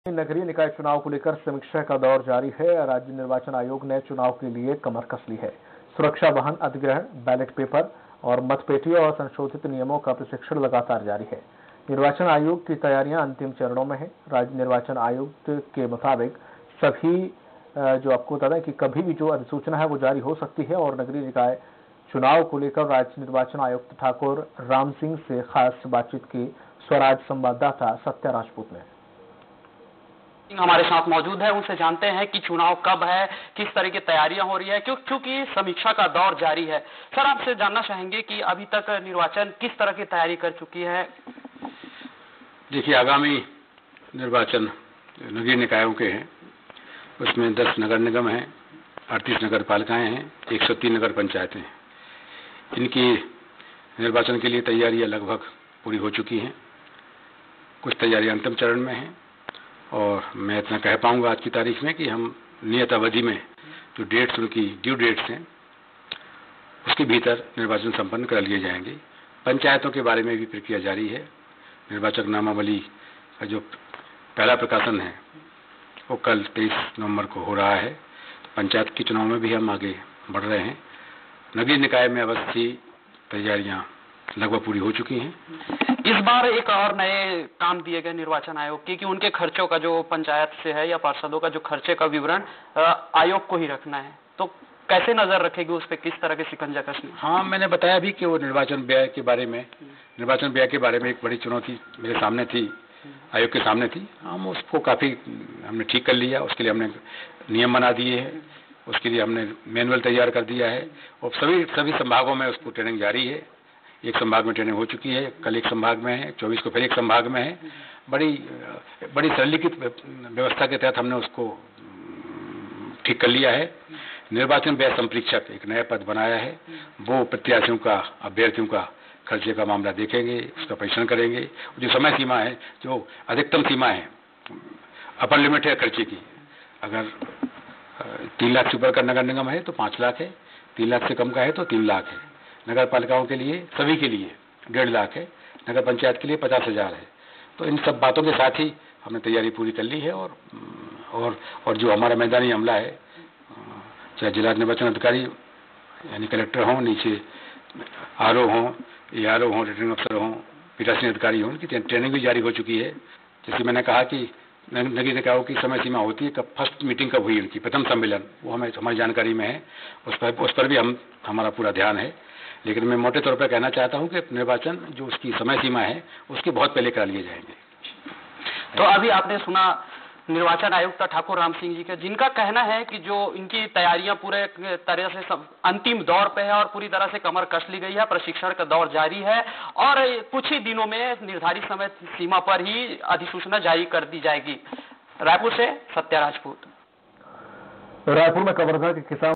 Raipur News CG: राज्य निर्वाचन आयुक्त Thakur Ram Singh से Swaraj Express की खास-बातचीत
Chief Electoral Officer, Chhattisgarh Satya Rajput